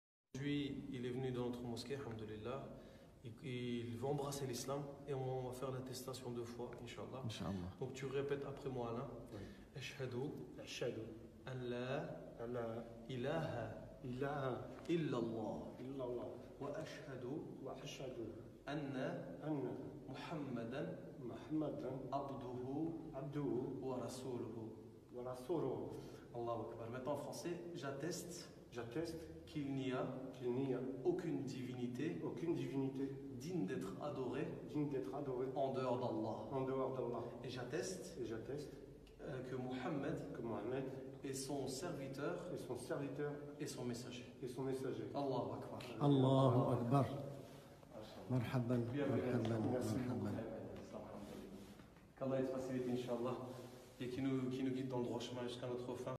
French Man Converts To Islam
A man converts to Islam in France.

Welcome Bilal 💚